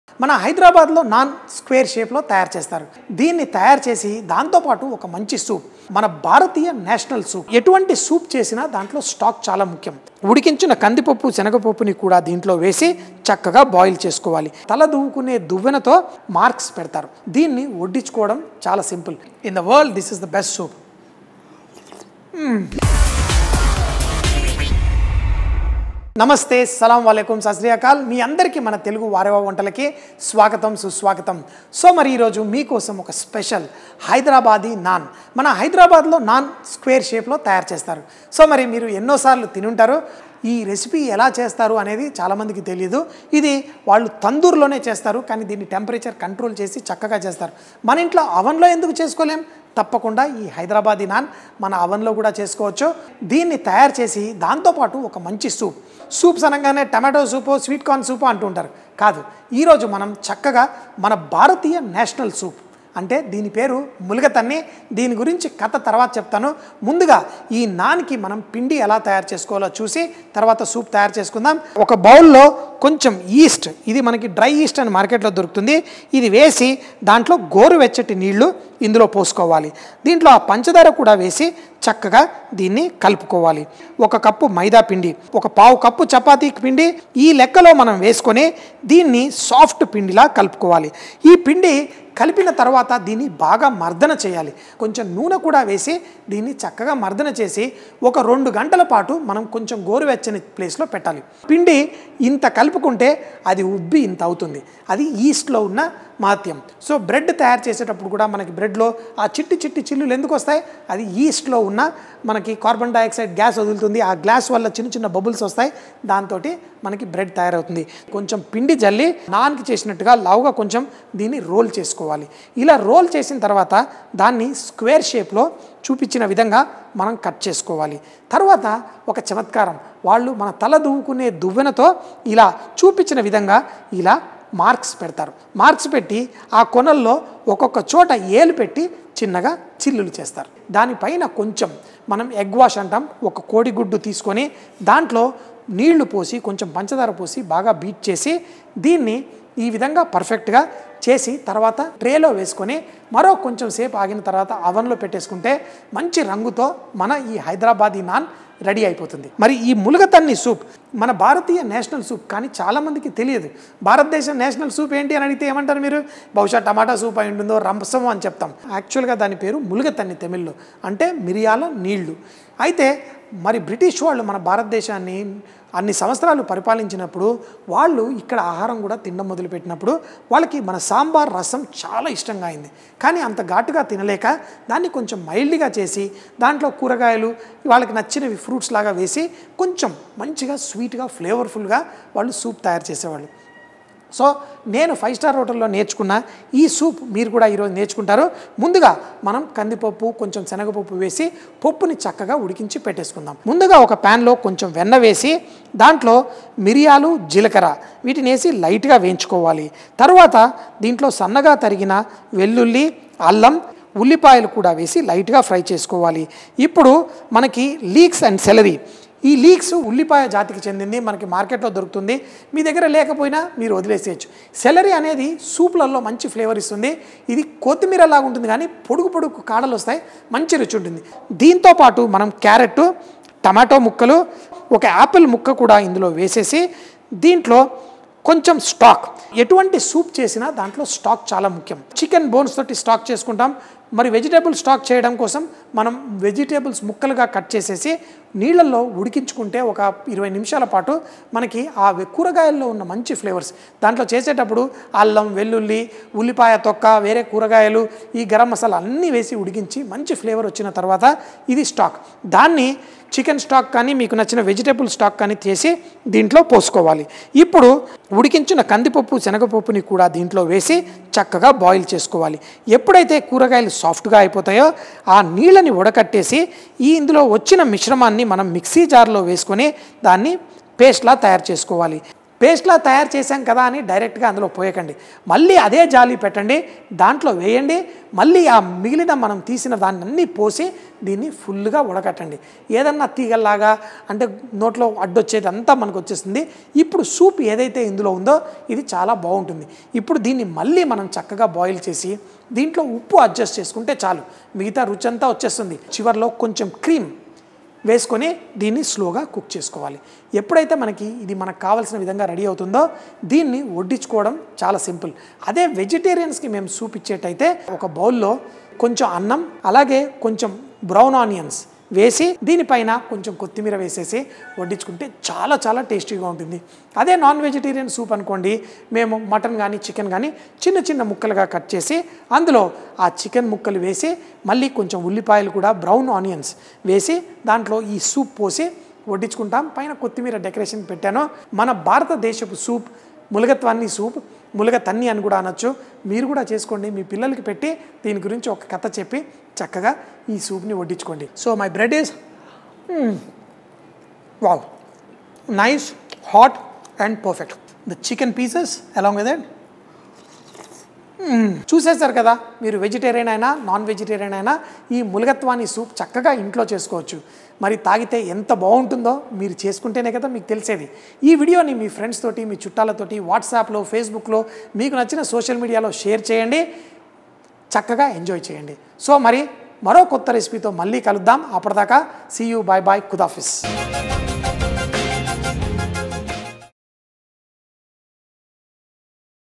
హైదరాబాదీ నాన్ విత్ ముల్గాతని సూప్ - Telugu Hyderabadi Naan with Indian National Soup Muligatwany
హైదరాబాద్ స్పెషల్ డిష్ అయిన నాన్ మరియు ఇండియన్ నేషనల్ సూప్ అయిన ముల్గాతని సూప్ ని ఇంట్లోనే సులభంగా ఎలా తయారు చేసుకొవాలో నేర్చుకుందాం.

హైదరాబాదీ నాన్ విత్  ముల్గాతని సూప్

Ingredients For Soup:-
Butter - 2 tb spn
Onions(chopped) - ½ cup
Leeks - ½ cup
Celery - ½ cup
Carrots - ½ cup
Tomatoes - ½ cup
Apple - ½ cup
Vegetable Stock - 1 liter
Toor dal(boiled) - ½ cup
Chana Dal (boiled) - ½ cup
Method:-
Heat butter in a pan add butter, pepper, cumin seeds and roast it for a minute, add garlic, ginger, onions. Sauté it for some time. Now add celery, carrots, tomatoes, chopped apple, vegetable stock, boiled toor dal, and chana dal boil it for some time. Drain it and grind it and again strain it as shown in the video.
Lastly, add salt and finish it by topping it up with fresh cream.

Hyderabadi Naan:-
Ingredients:-
Egg Wash - 2 tsp
Method:-
In a Mixing bowl add yeast, warm water, sugar and activate the yeast. Then add Maida, Wheat flour and mix it into a soft dough.
Rest it for 2-3 hours in a warm place.
Now take the dough and roll it with a rolling pin & cut it into squares shape.
Give it an egg wash and bake it in the oven for 12-15 minutes.

Morning  - Vegetarian
Evening  - Non Veg